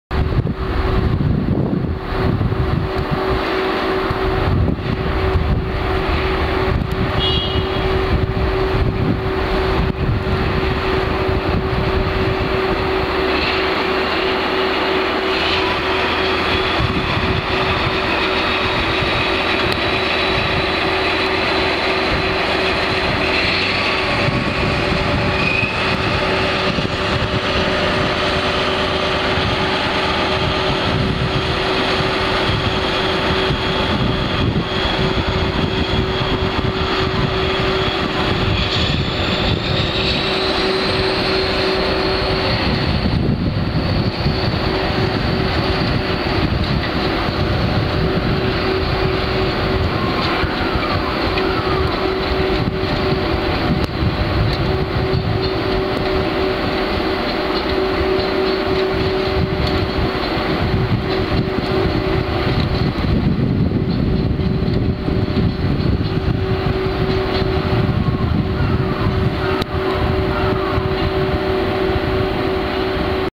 ３０８１レ高岡駅出発
ＤＥ１０ １５９８牽引城端線二塚行き貨物列車３０８１レの、高岡駅出発の様
子。